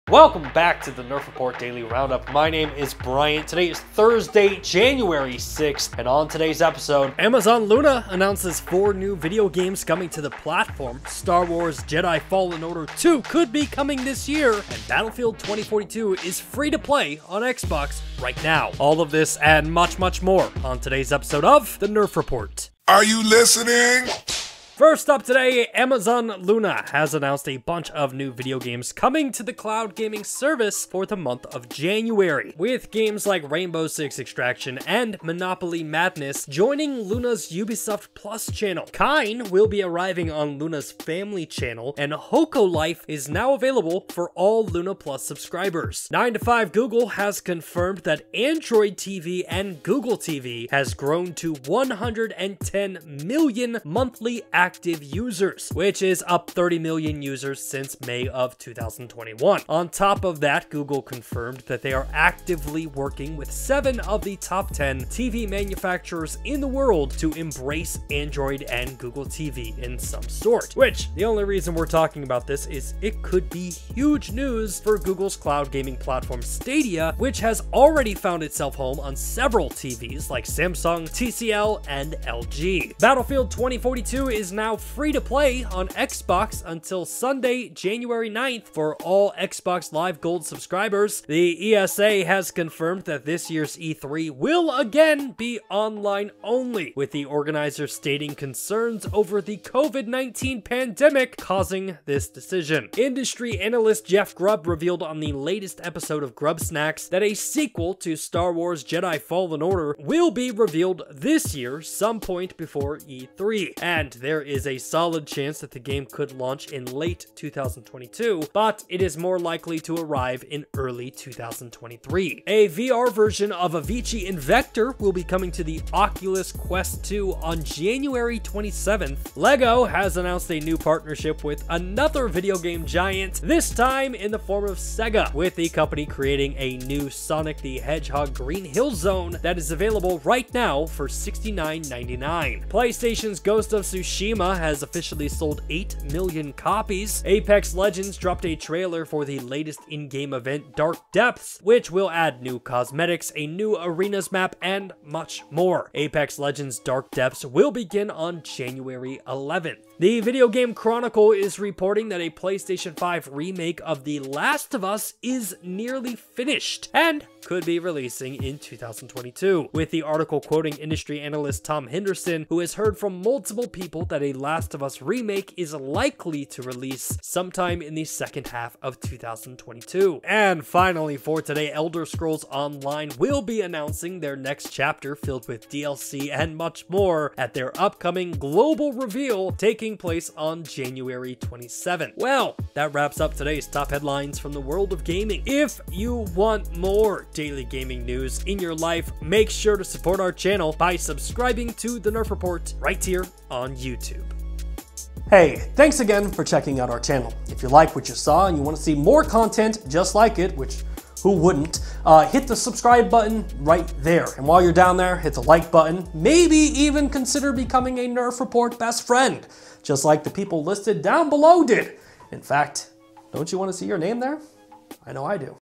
Amazon Luna Adding 4 New Games + Star Wars Jedi Fallen Order 2 News + Battlefield 2042 Free To Play
On today's episode we dive into Amazon Luna announcing it's January lineup, Star Wars Jedi Fallen Order 2 arriving this year, and Battlefield 2042 being free for Xbox players.